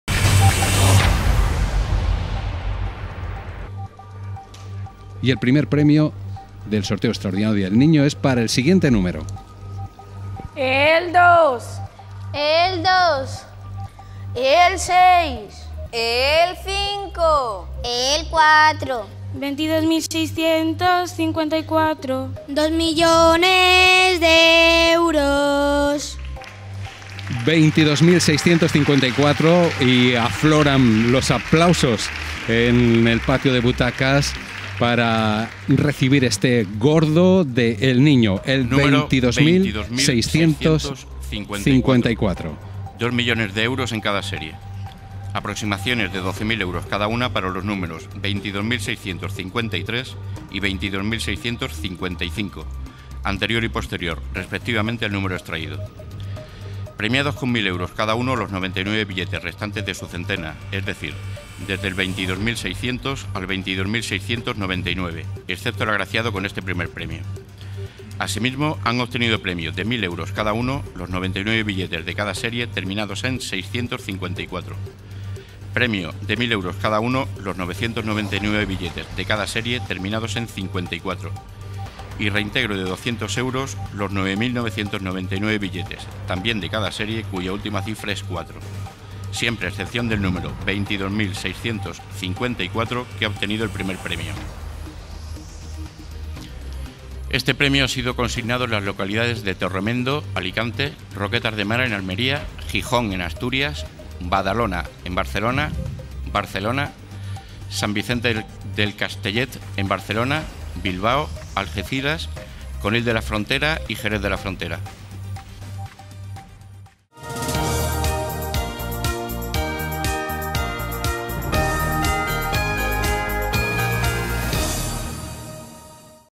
22.654 PREMIO GORDO SORTEO DEL NIÑO 2016 - (2.000.000 EUROS)
El premio Gordo de la lotería del Niño de 2016, dotado con 90 millones de euros (200.000 al décimo) ha sido para el 22.654, ha estado muy repartido y se ha vendido entre otras provincias, en Madrid, Barcelona, Alicante, Asturias, Bilbao y Cádiz.

Entre las localidades afortunadas con este primer premio están Torremendo (Alicante), Roquetas del Mar (Almería), Gijón (Asturias), Badalona (Barcelona), Barcelona, San Vicente del Castellet (Barcelona), Bilbao, en la localidad de Alhama de Murcia y los municipios gaditanos de Algeciras, Conil de la Frontera, Jerez de la Frontera.

El segundo premio de este sorteo extraordinario ha sido para el 60.755, vendido en Madrid, y tercero ha correspondido al 95.395, consignado en la localidad de Guardo, en Palencia.

Los números terminados en 38, 36, 93, 14 y 04 están premiados con 40 euros al décimo y los reintegros han correspondido a las terminaciones en 4, en 0 y en 2.

630 millones en premios
El sorteo del Niño reparte en total 630 millones de euros, 70 millones más que el año anterior. Además del primer premio se reparten un segundo de 33,75 millones (75.000 al décimo) y un tercero de 11,25 millones (25.000 al décimo). Además hay 20 premios de 3.500 euros cada uno, 1.400 premios de 1.000 euros y 5.000 premios de 400 euros.

Una de las novedades del sorteo este año es que ha salido de su emplazamiento habitual, en el salón de Sorteos de Loterías, para celebrarse en el Círculo de Bellas de Artes de Madrid, donde la entrada será libre hasta completar el aforo.

Desde 1908, el sorteo ha agraciado con su primer premio a Madrid hasta en 40 ocasiones. Tras la ciudad de Madrid, las ciudades más premiadas en más de un siglo de historia del sorteo son Barcelona, donde el primer premio del Niño ha caído hasta 34 veces, Bilbao (16), Valencia (14) y Sevilla (10).

A diferencia del de Navidad, este sorteo se celebra mediante el sistema de bombos múltiples y dura treinta minutos; además, sigue un guión preestablecido, por lo que no genera tanta incertidumbre.